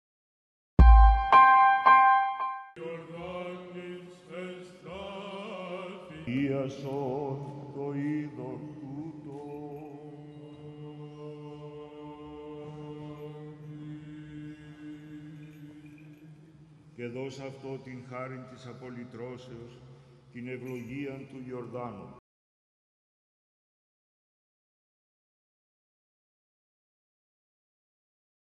H λειτουργία των Θεοφανείων στο Οικουμενικό Πατριαρχείο Κωνσταντινουπόλεως
H λειτουργία των Θεοφανείων στο Οικουμενικό Πατριαρχείο Κωνσταντινουπόλεως. ΕΡΤ/ΑΠΕ-ΜΠΕ